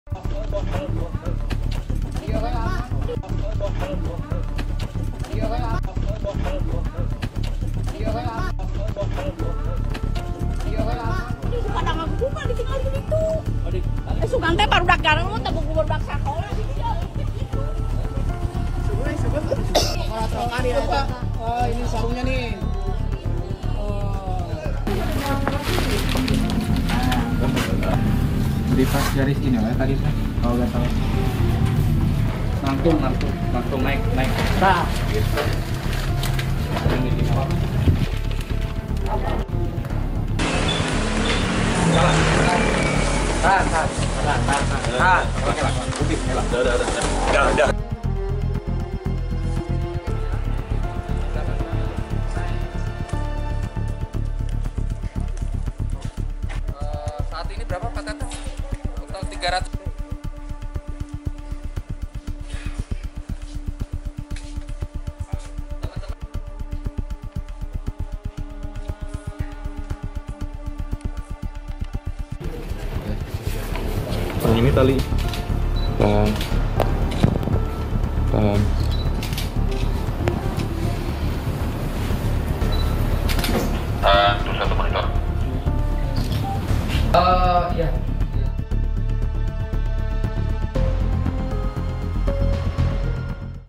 2 Napi Ditangkap Warga Usai Kabur dari Lapas
2 orang narapidana Lapas kelas 2b Sumedang melarikan diri dengan memanjat dinding pagar lapas, Selasa siang.

Pelarian keduanya berhasil digagalkan petugas yang dibantu warga.